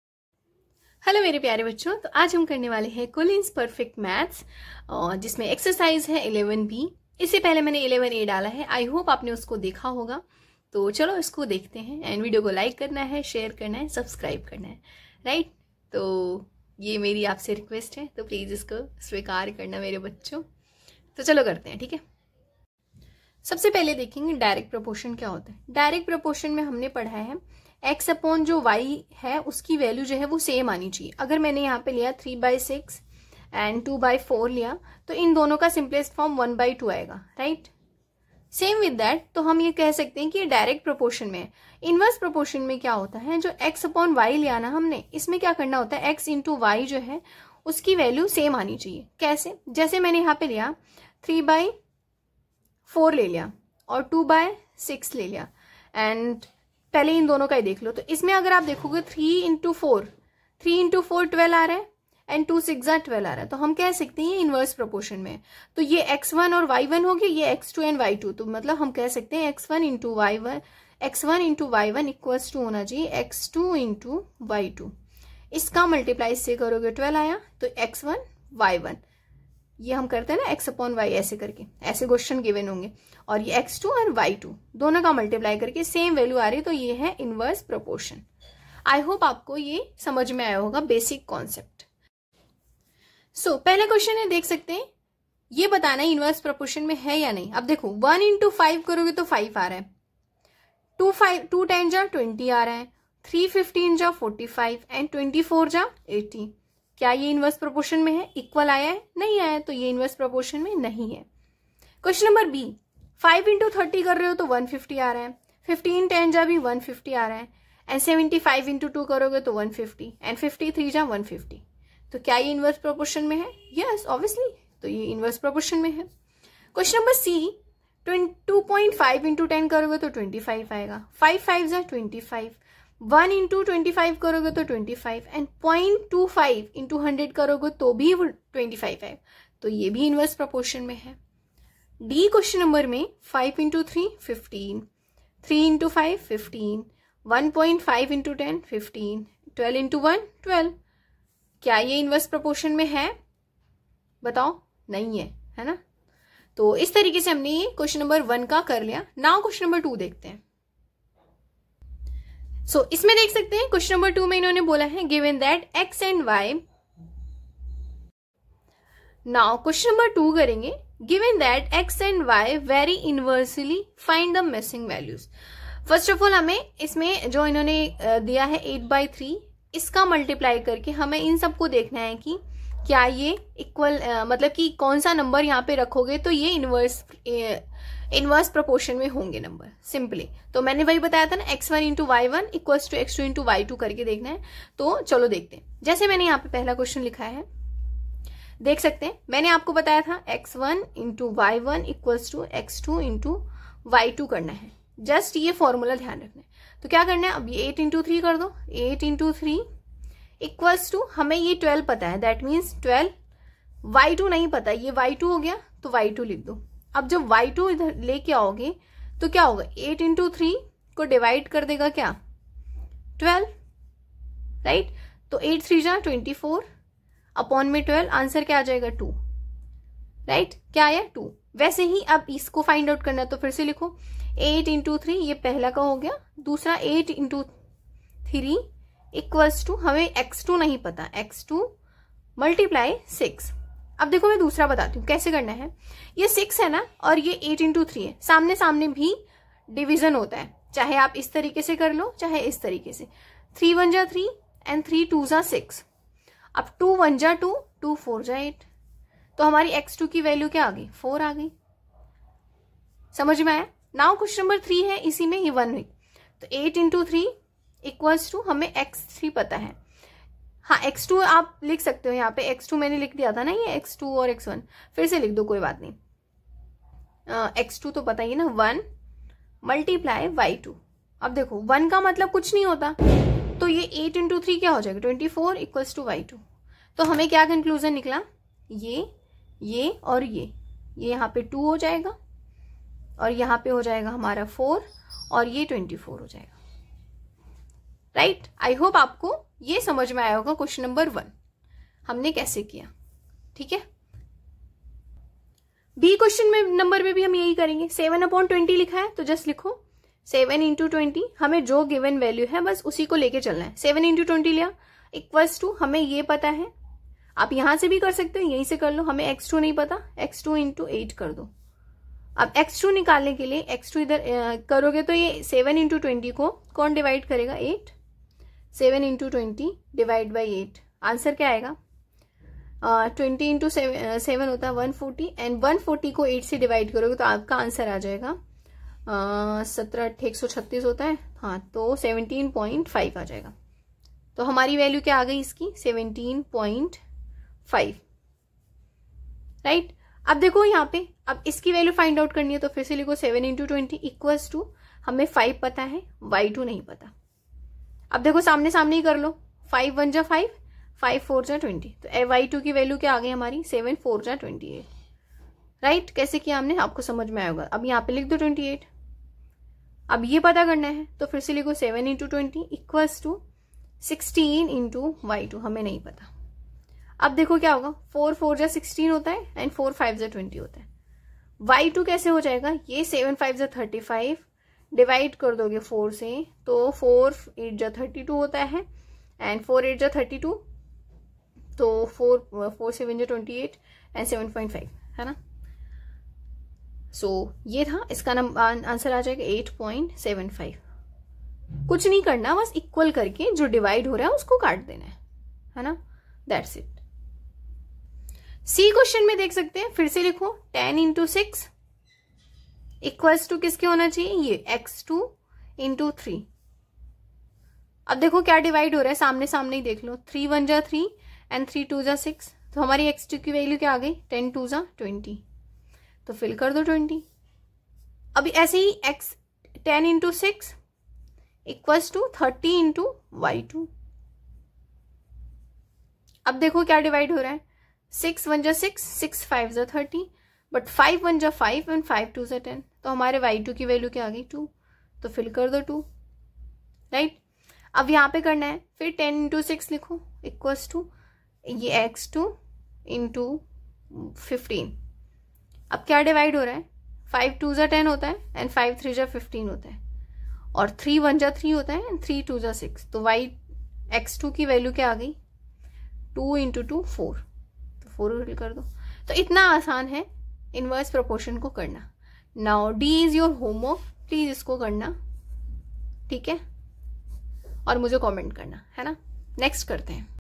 Ch-11b,Direct and Inverse Proportions Collins Perfect maths class 8
Direct proportion: When two variables are directly proportional, it means that as one variable increases, the other also increases in a consistent and proportional manner. For example, if you double the amount of a particular ingredient in a recipe, the yield of the dish will also double.

Inverse proportion: Inverse proportion occurs when one variable increases while the other decreases in a consistent and proportional manner. An example is the relationship between the time it takes to complete a task and the number of people working on it – as more people are assigned to the task, the time it takes to complete it decreases.

If you have any questions regarding this please comment I will help you!

All the best bachee!!